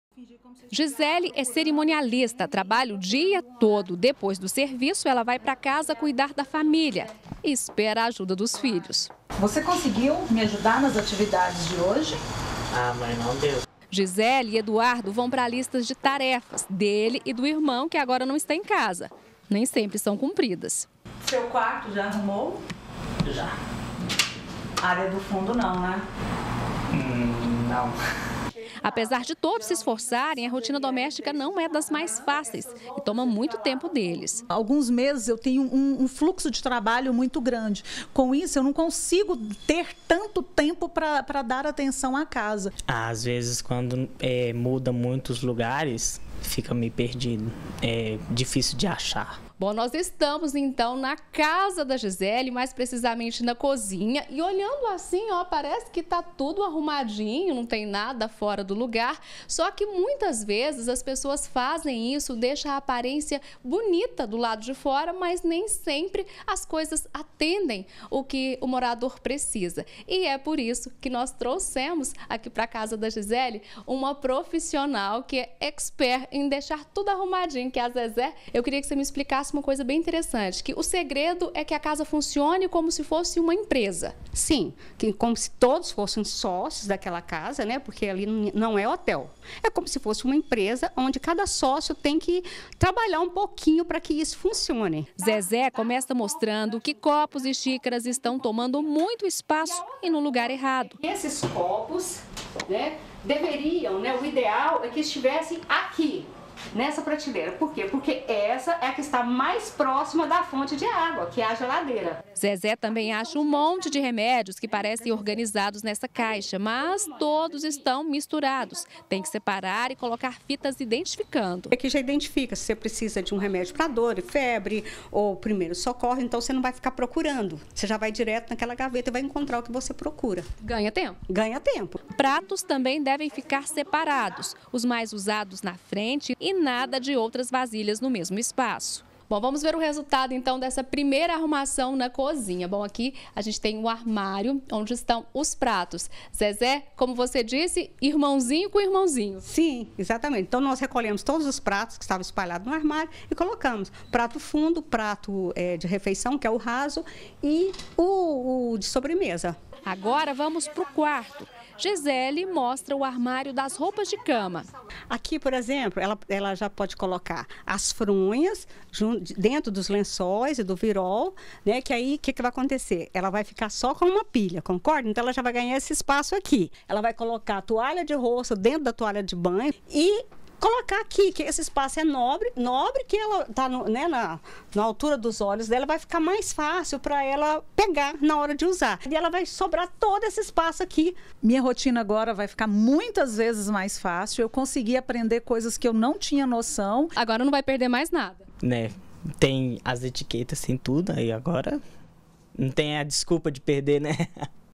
GR - Gerenciamento de casa: Como facilitar as tarefas de casa - 13-06-2018
Na correria do dia a dia sobra pouco tempo para arrumar as coisas em casa. E se tudo não estiver no lugar, a situação fica ainda mais difícil. Pois é, algumas medidas simples e de organização ajudam a ganhar tempo.